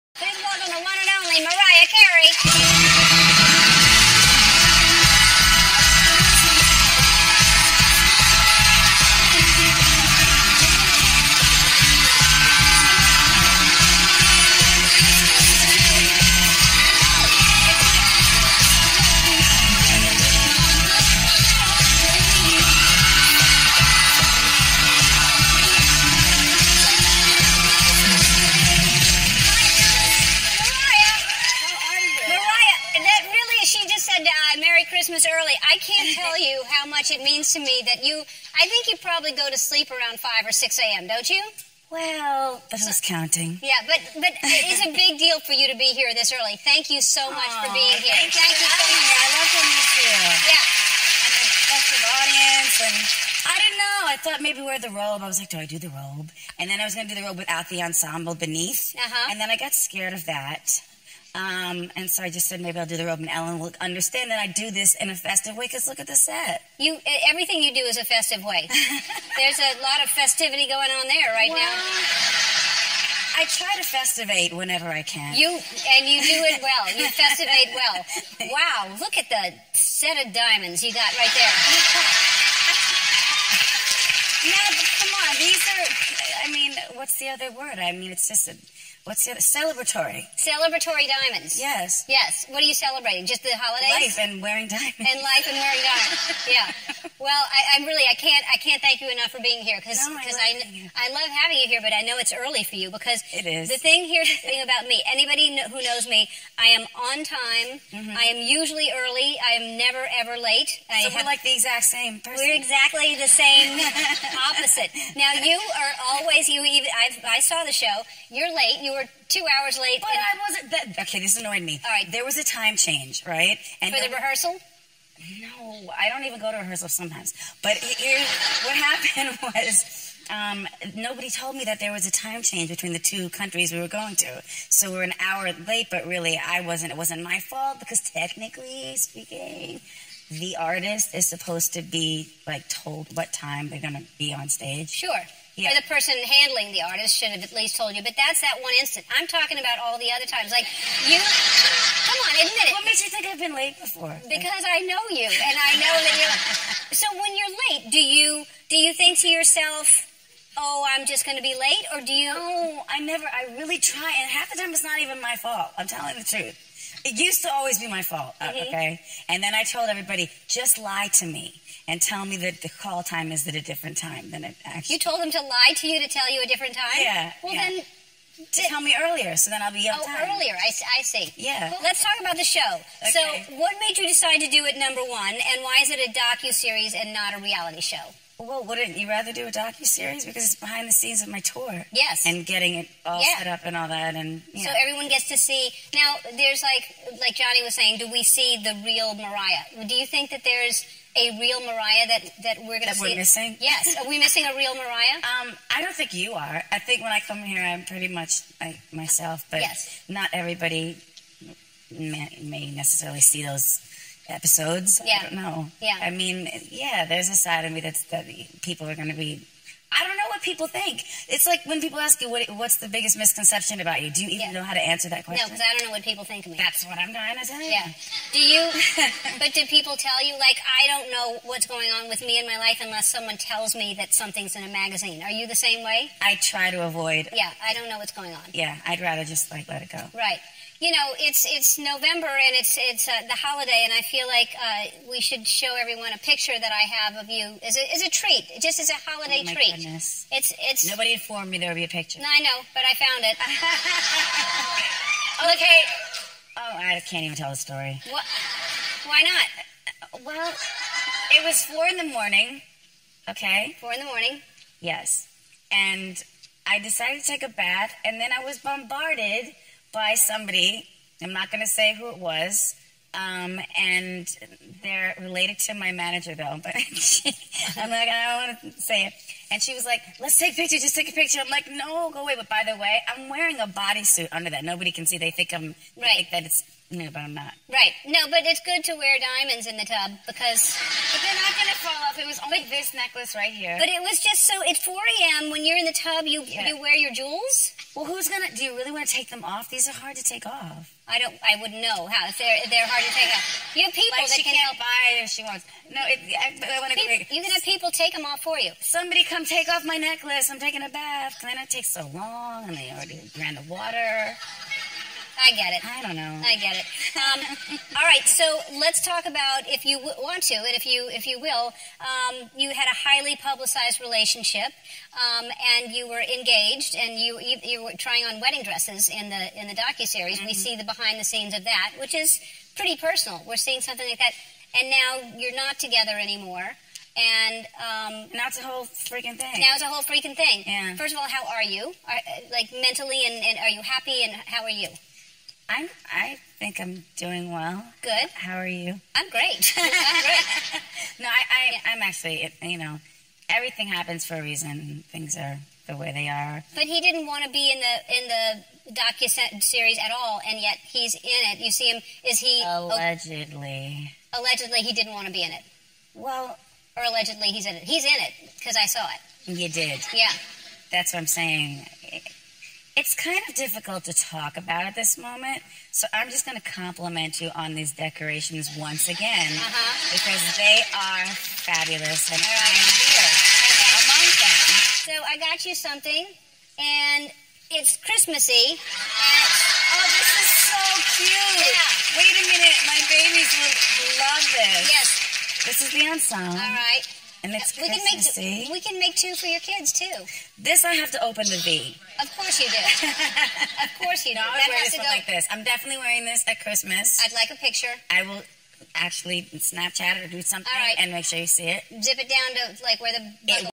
Mariah Carey Brings the Holiday Glamour - TV SHOW KING
You never know what the singing superstar's visit will bring. This time she brought an impressive set of… bling!